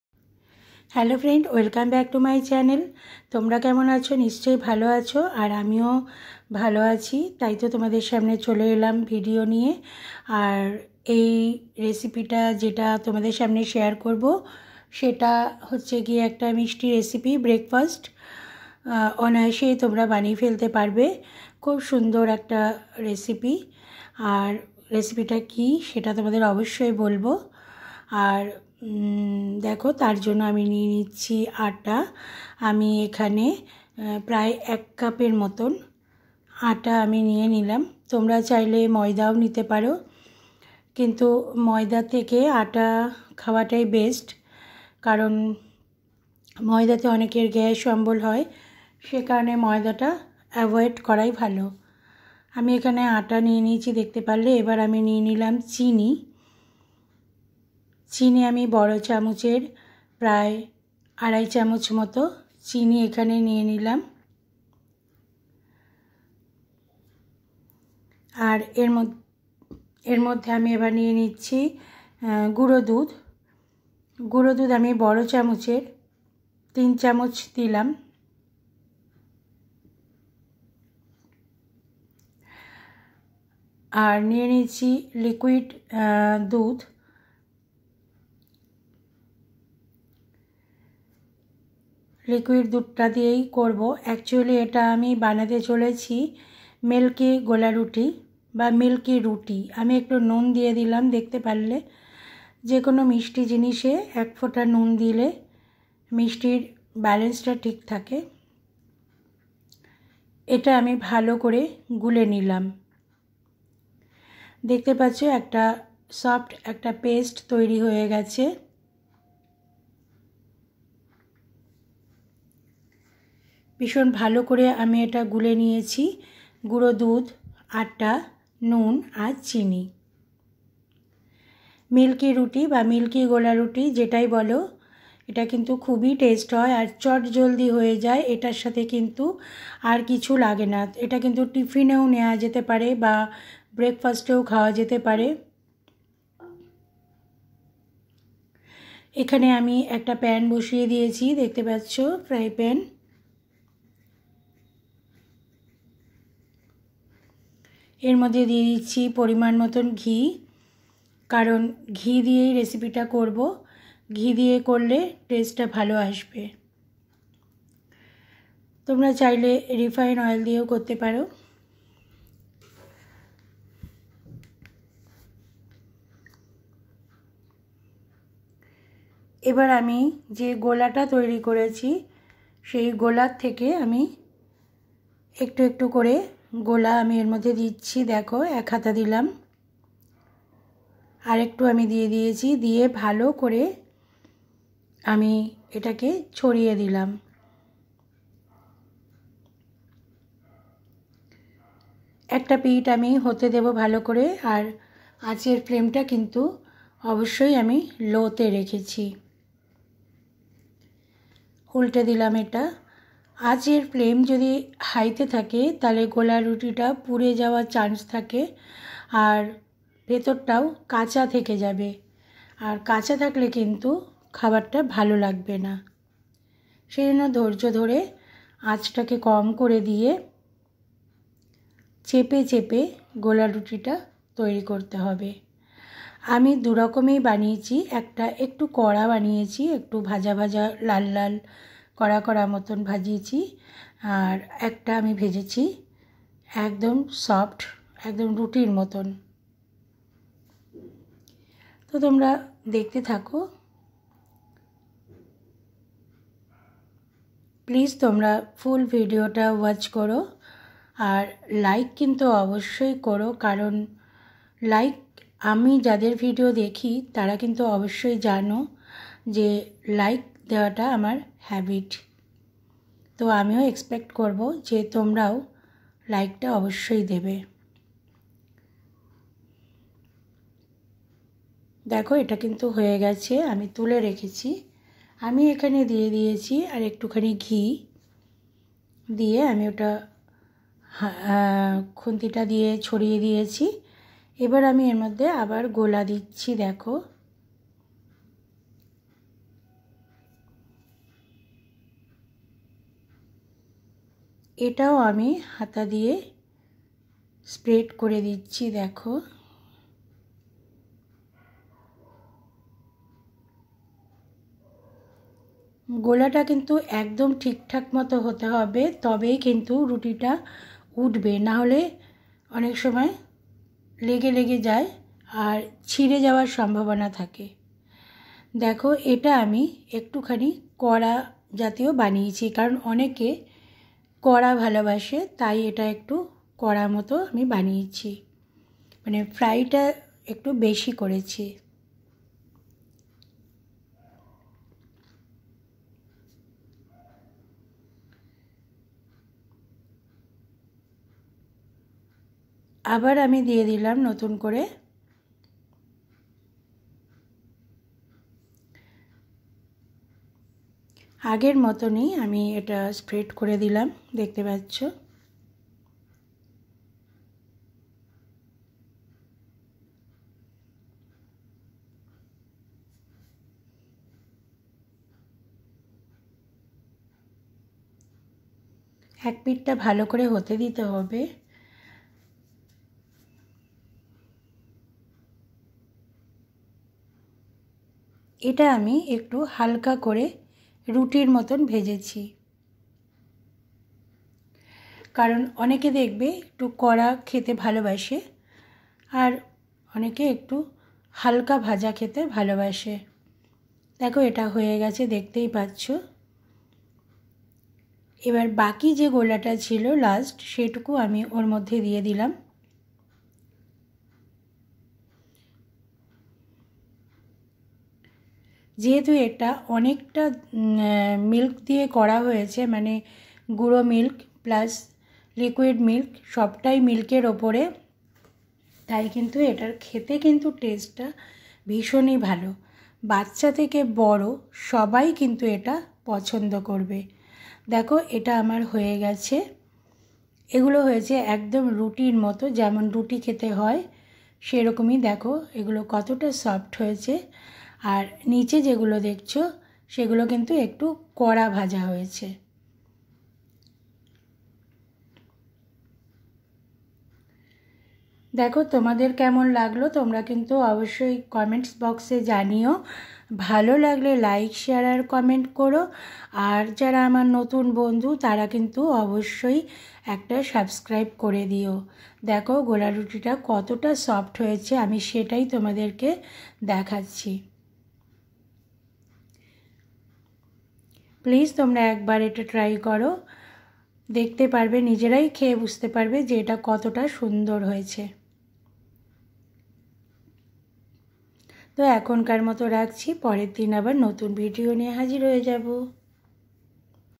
Milk golaruti Recipe / Crispy milk Paratha /Breakfast Recipe /Milky ruti#RitasDiary
Milk Paratha Recipe | Crispy milk golaruti | Delicious parata | Breakfast Recipe

Ingredients

1.Atta
2.Milk powder
3.liquid milk
4.Salt one spinch
5.Sugar
6.Ghee